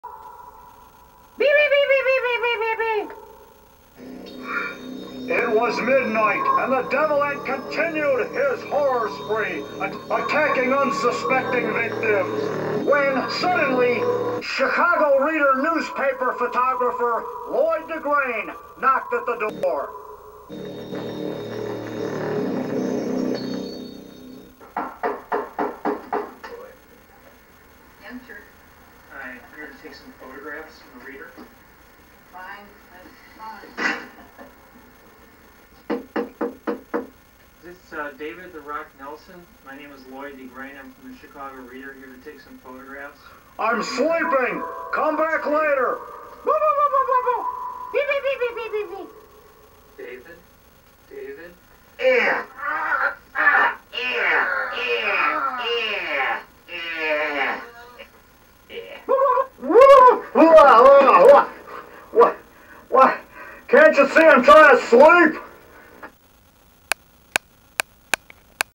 David Rock Nelson's "Devil Ant 4"! Chicago Reader photographer Lloyd DeGrane meets Devil Ant!
I video-edited this scene into my "Devil Ant -4" movie lastnite, in my basement a.k.a. "Dungeon", finishing at 9:01 pm! B-"Monster" Moviemaker, David Rock Nelson's "Devil Ant 4"! SEE: "Chicago Reader" Newspaper photographer Lloyd DeGrane meet the fearsome, legendary Devil Ant! SEE: The Director, David Rock Nelson(as Detective Rock), sleeping & "snoring" like Curly & Shemp! You've Never seen Anything like it! SEE: David Rock Nelson's "DEVIL ANT 4: Devil Ant Invades Vegas"! Coming SOON, in 2015, to a dvd player near you! Your "Fiend", Rock!